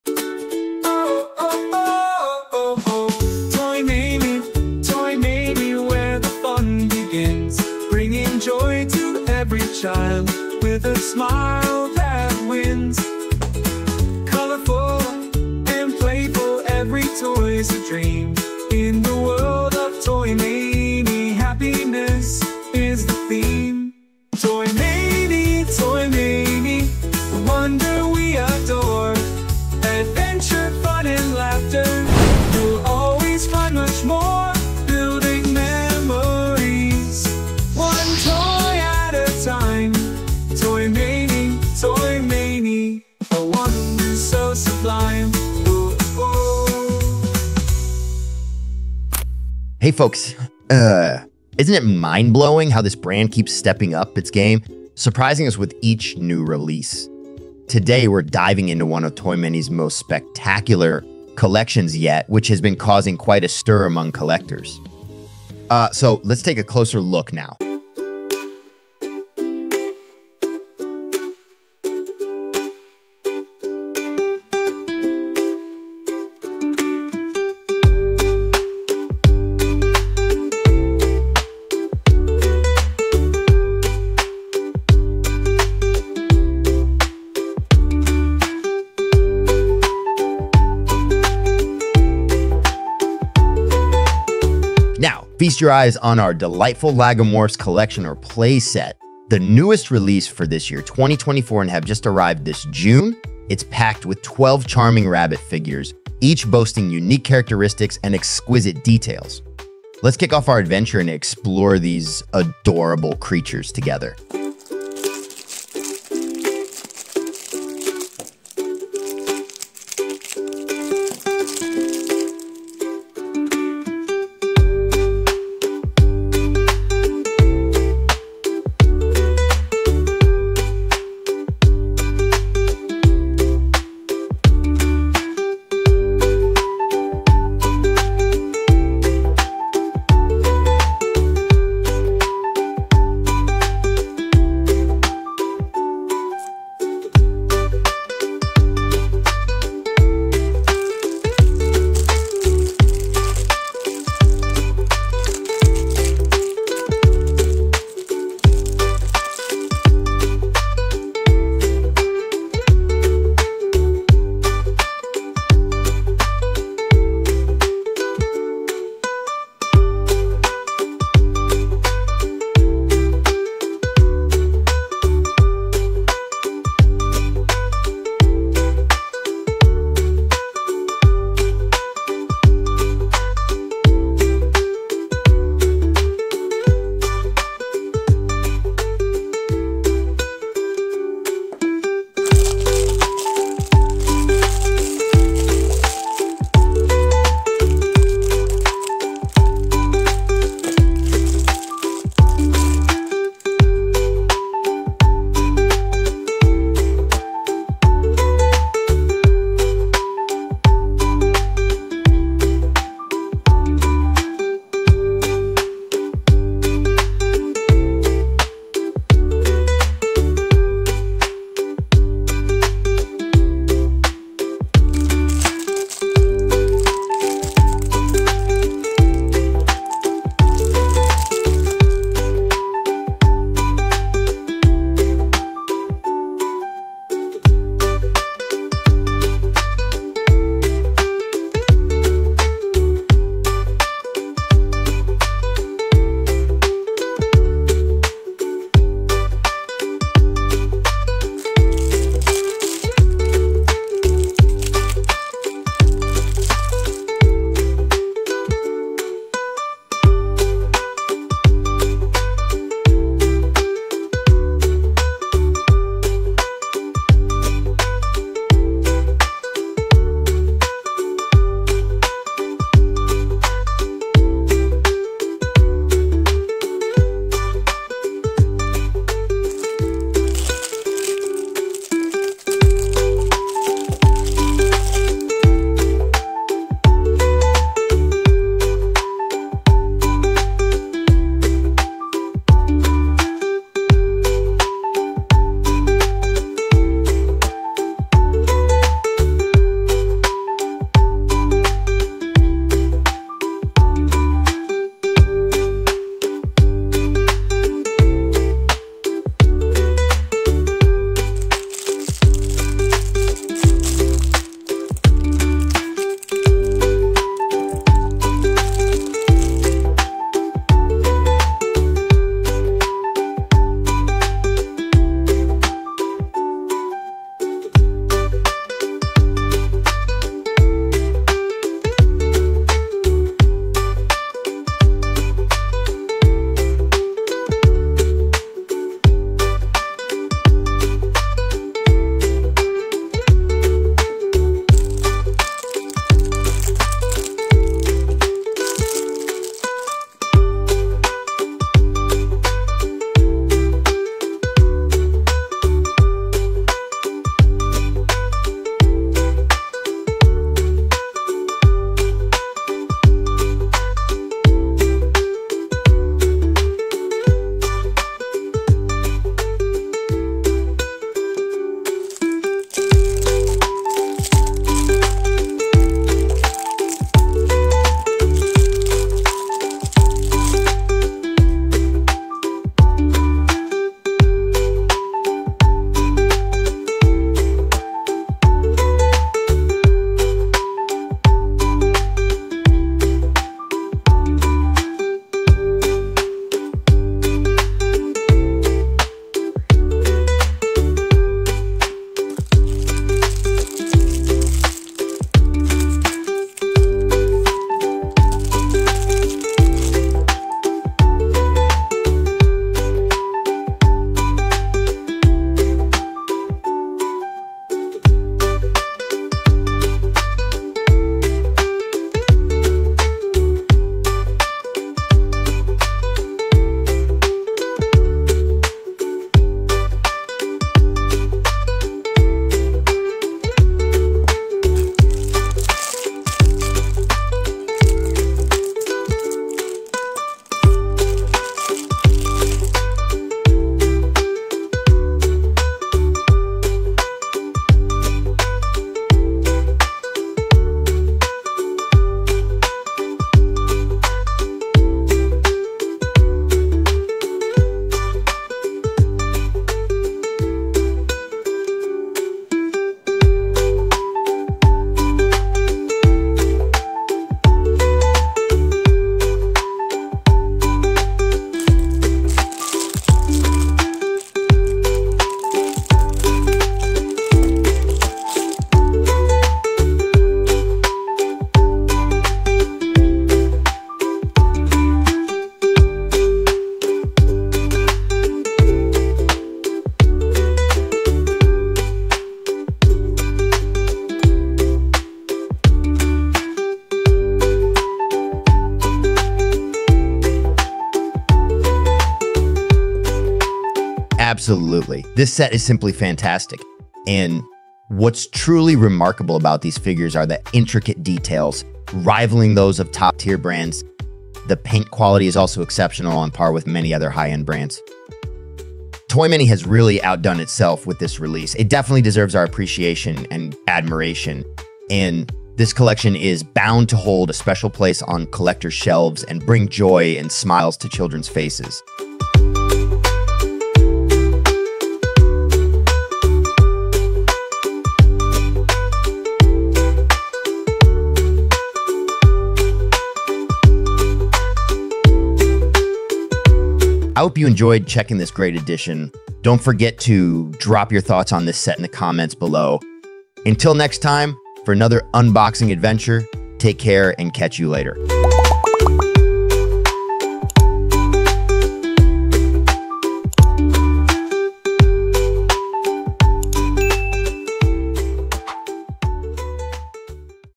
The Best Rabbit Figures? Toymany's New release !!!!😍
Join me as i unbox and explore Toymany's stunning lagomorphs collection! a new release set features 12 beautifully detailed rabbit figures that rival top-tier brands in quality and craftsmanship. Perfect for collectors and kids alike, these adorable figures are sure to bring joy and smiles. Watch my in-depth preview to see why this collection deserves a special place on your shelf!